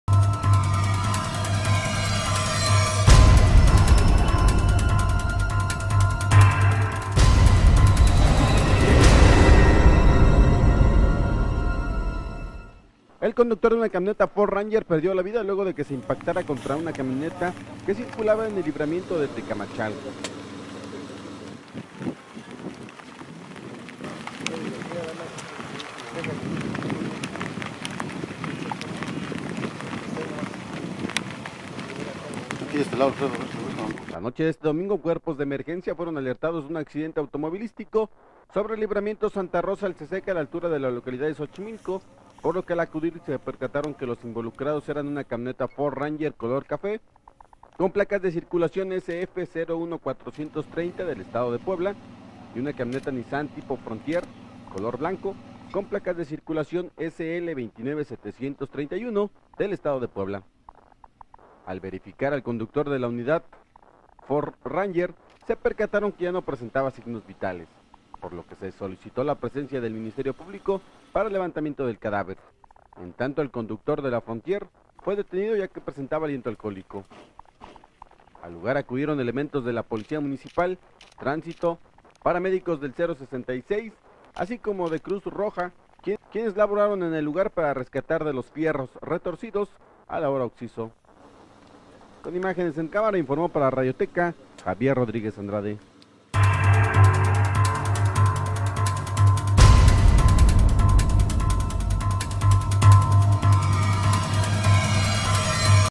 Una persona pierde la vida en accidente automovilístico
Una persona perdió la vida en un accidente automovilístico en el municipio de Tecamachalco, al lugar acudieron cuerpos de emergencia.